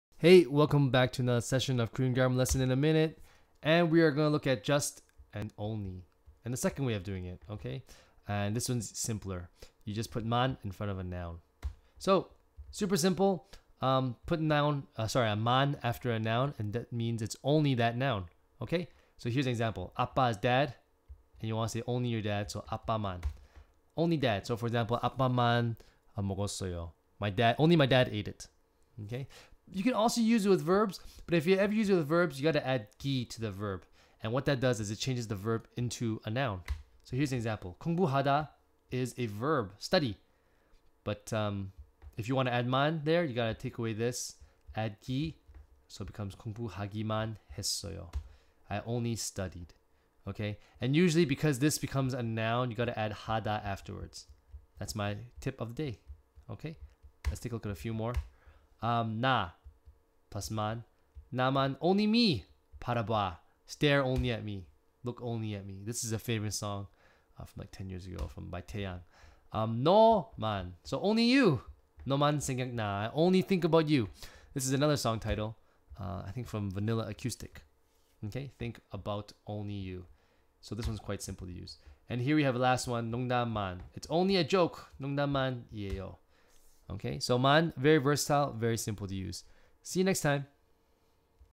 Korean Grammar in One Minute - Just/Only #2 (N + 만)
Learn how to say Just/Only in Korean. This one you can use only with nouns

1. Add 만 to a noun
2. If you want to use it with a verb, add 기 to the verb (don't forget to finish the sentence with another verb though)

너 + 만 = 너만 사랑해요                                     Only love you
공부하다 + 기 + 만 = 공부하기만 해요               Only study
농구만 + 만 = 농구만 해요                                 Only play basketball